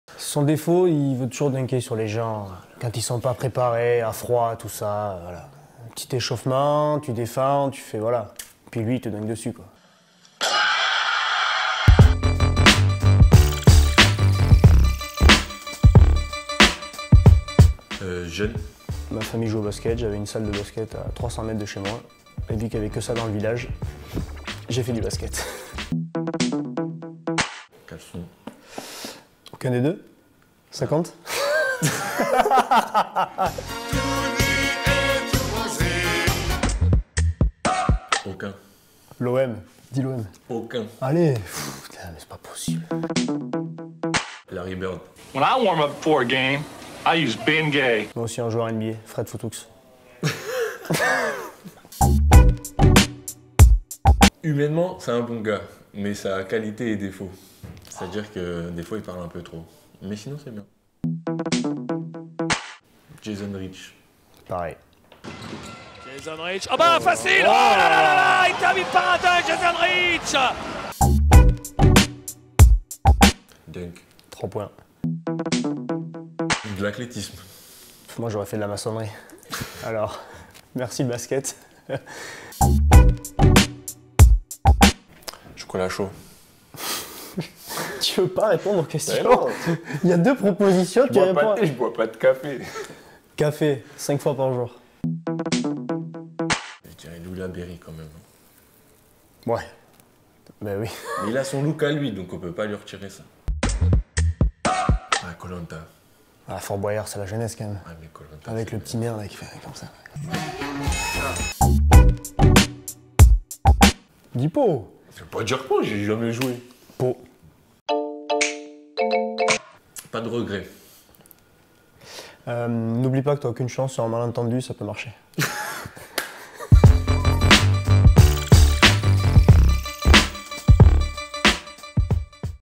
1 Contre 1 | Rémi Lesca vs Maxime Roos
Rémi Lesca en maçon, les débuts de Maxime Roos, la qualité de l'un, le défaut de l'autre...
Rémi vu par Maxime, Maxime vu par Rémi...
 C'est l'heure d'un nouvel 1 contre 1 !

 LNB / Tommy Hombert